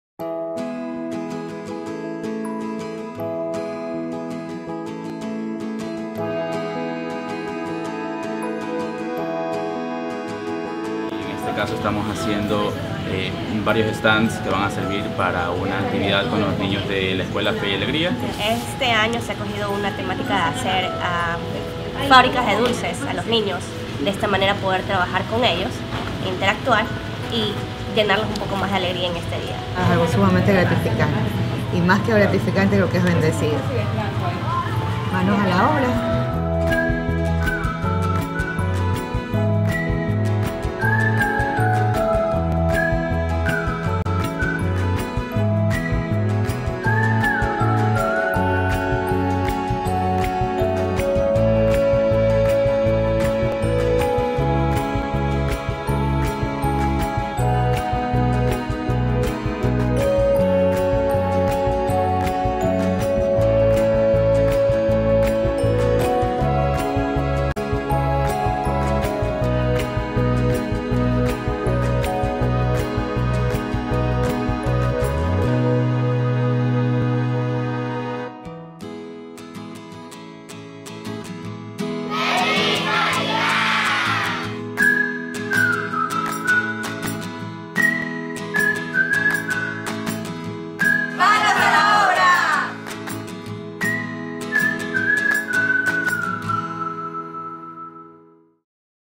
Manos a la Obra
En esta Navidad nos pusimos manos a la obra, preparamos una fábrica de dulces en nuestras oficinas.